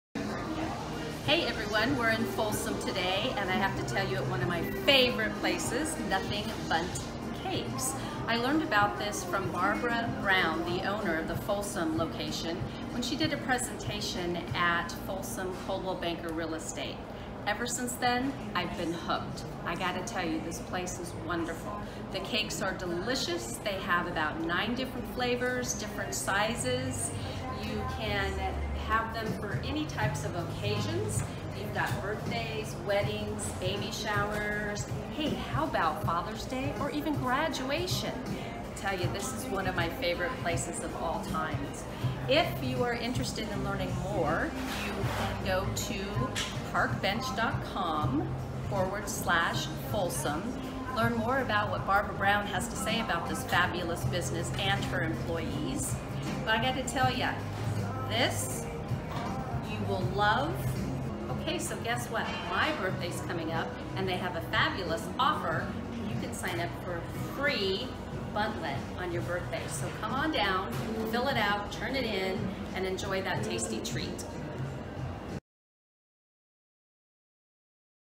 Nothing Bundt Cakes - Folsom
Fun and off the chart Bundt Cakes interview, You will never think of Bundt Cakes the same.